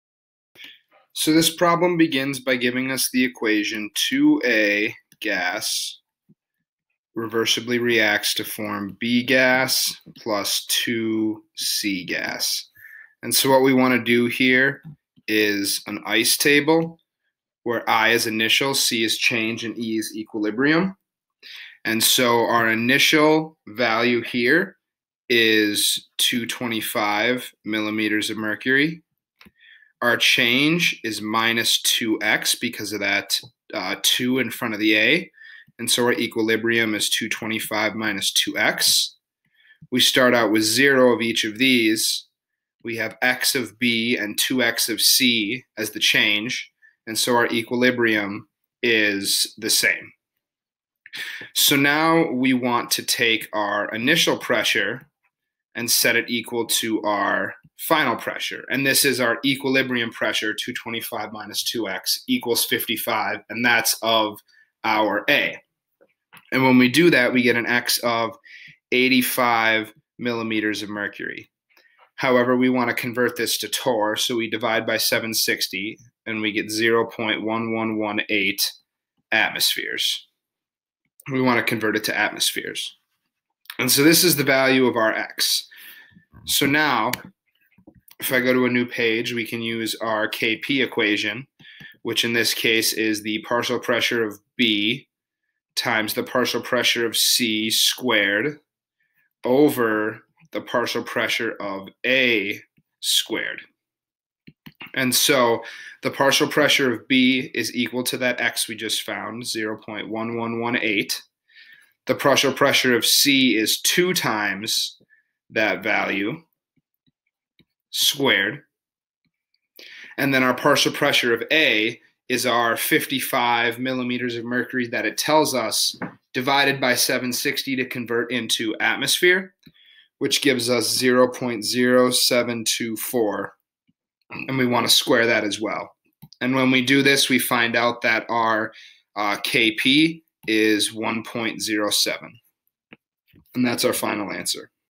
For the reaction 2 Ag Bg + 2 Cg, a reaction vessel initially contains only A at a pressure of PA = 2
For the reaction 2 A(g) B(g) + 2 C(g), a reaction vessel initially contains only A at a pressure of PA = 225 mmHg. At equilibrium, PA = 55 mmHg. Calculate the value of Kp. (Assume no changes in volume or temperature.)